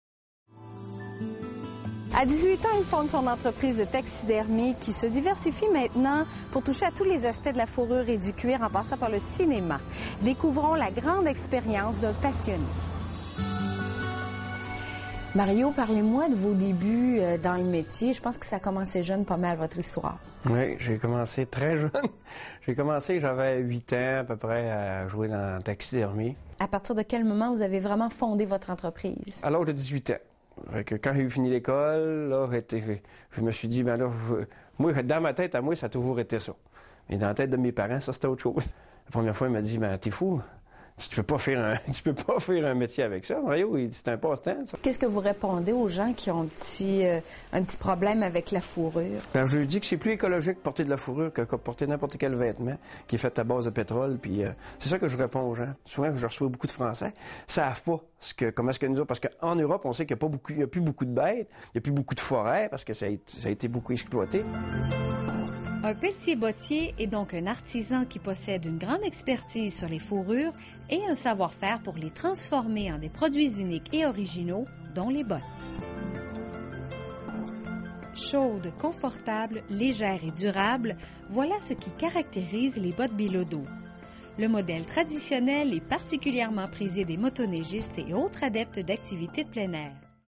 ÉCONOMUSÉE du pelletier-bottier, Bilodeau inc, Normandin (Lac Saint-Jean)
Venez découvrir les facette du métier de pelletier-bottier à l'ÉCONOMUSÉE. Une aventure qui vous plongera dans le monde de la fourrure, de la faune, de la botte et de l'hiver québécois.